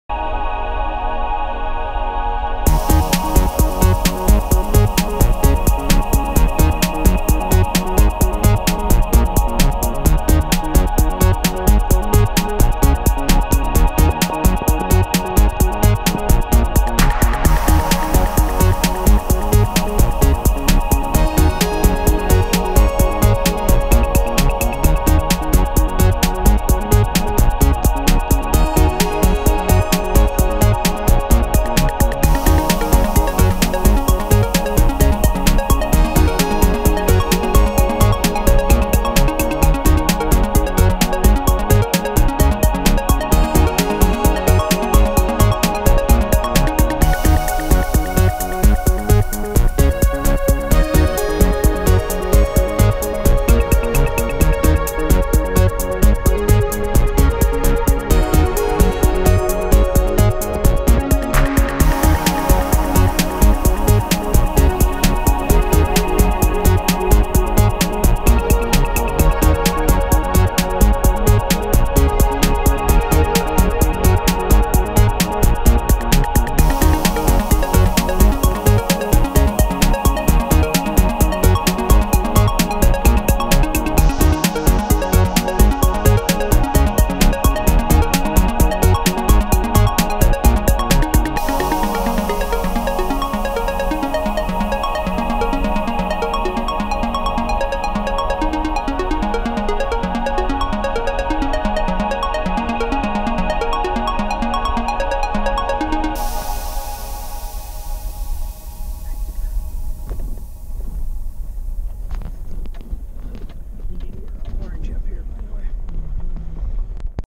1. To the top of Timberwolf Mountain [NATURE]
A juxtapostion of nature, a great music track, visual effects,  and diverse quotes make this video. Approaching the summit of Timberwolf Mountain, a Washington Secret Place. Amazingly it is accessible by vehicle,.   This clip is the first segment in a longer video.